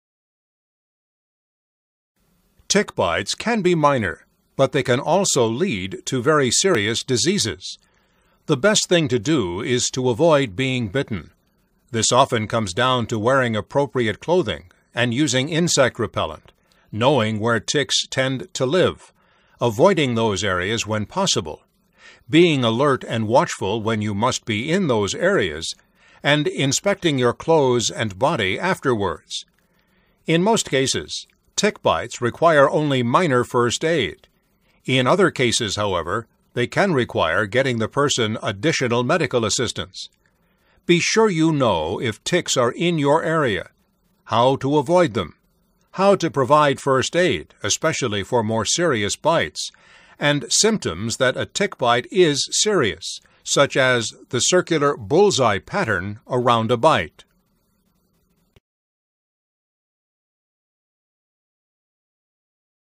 First Aid - Tick Bites Training
to view the full video and purchase access to our other Health & Safety (EHS) courses.

Ticks are small insects commonly found in grassy areas pretty much everywhere in the United States. They bite people and suck their blood; while doing so, they can transmit many dangerous diseases to the person they’re biting, with Lyme disease being the most notable. In this course, you’ll learn what a tick looks like and where ticks live; how to avoid being bitten by a tick; how to inspect your body for ticks; how to remove a tick from your body if you have been bitten; first aid for tick bites; symptoms of tick bites and serious reactions to tick bites; and tips for seeking medical care after a tick bite.